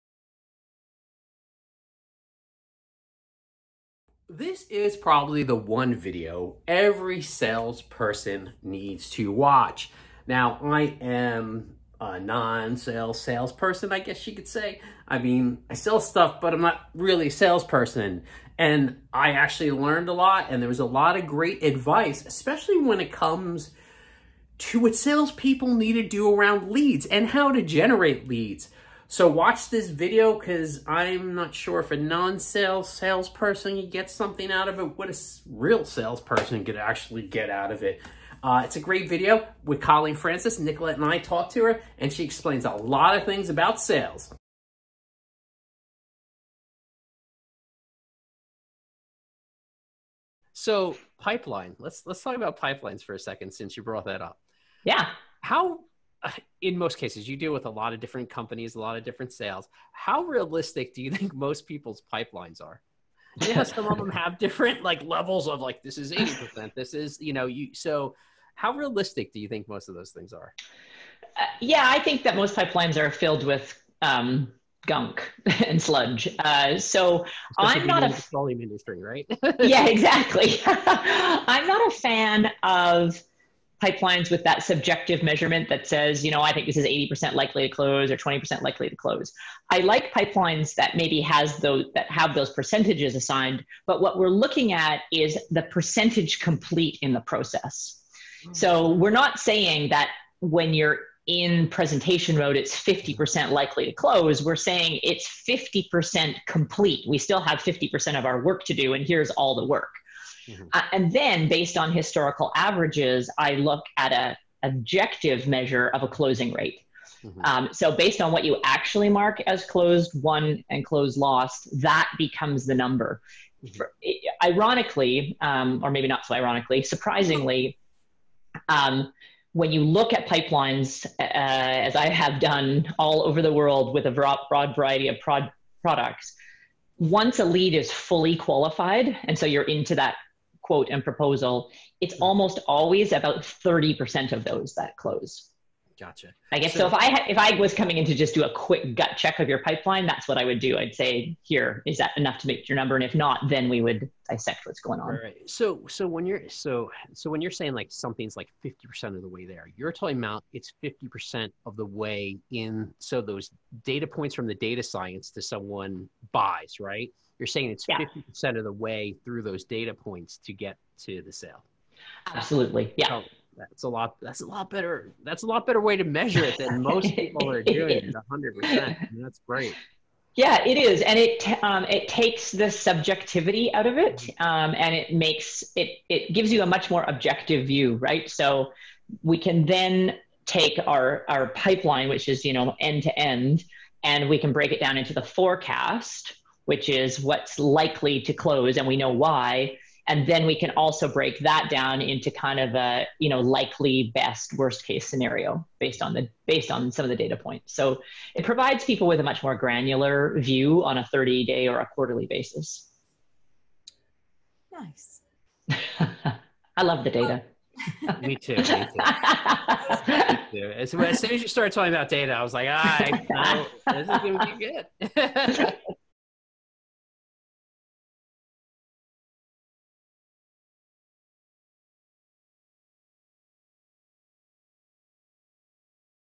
How to Create a REAL Sales Pipeline that WORKS!
Is your sales pipeline a work on fiction? How do you create a real sales pipeline that works! Colleen Francis, President of Engage Selling, explains how to set a sales pipeline!

Colleen's Books:
Nonstop Sales Boom: Powerful Strategies to Drive Consistent Growth Year After Year
Honesty Sells: How To Make More Money and Increase Business Profits